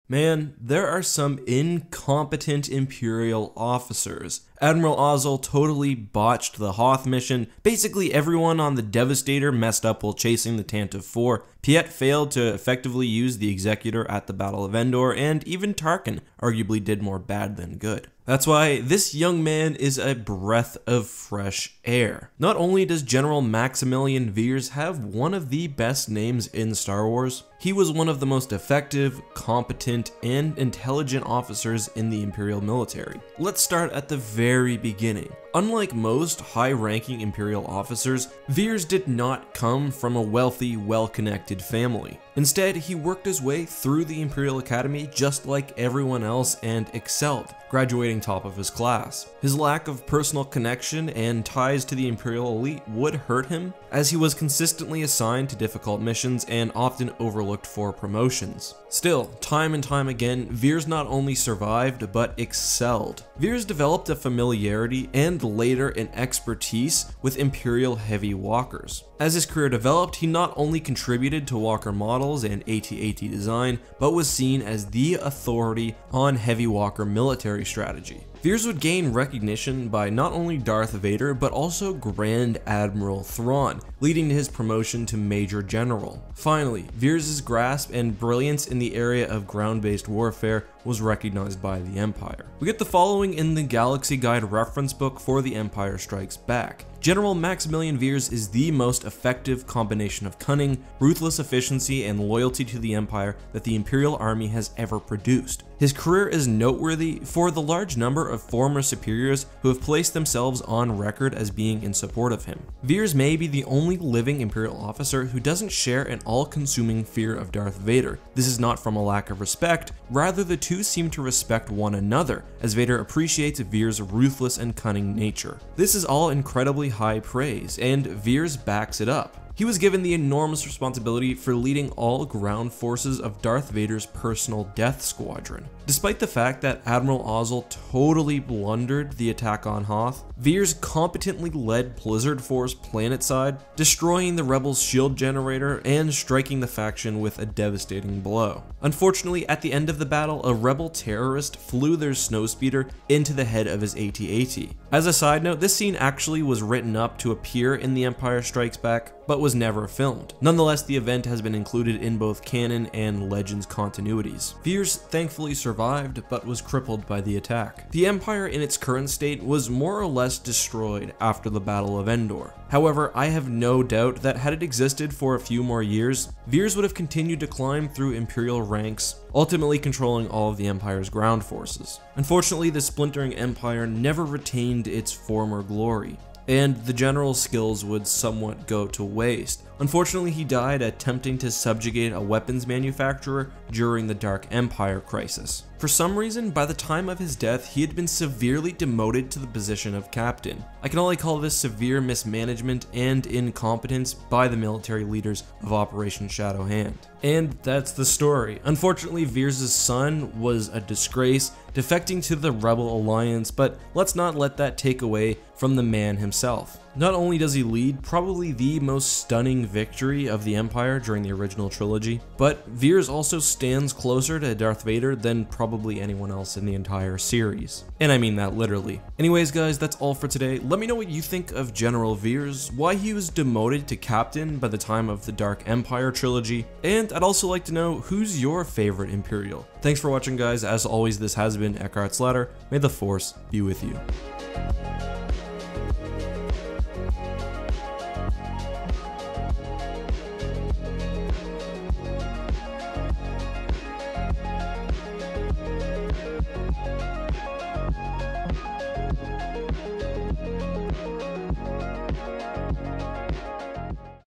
The GREATEST IMPERIAL OFFICER of all time | Star Wars Legends Lore
On today's Star Wars Legends Lore video, we take a closer look at the Greatest Imperial Officer ever (not named Thrawn).

The Galactic Empire was filled to the brim with incompetent officers. However, a few stood out, chief among them was General Maximilian Veers. Veers appeared first in the Empire Strikes Back during the Battle of Hoth, but also had an extensive backstory in Star Wars Legends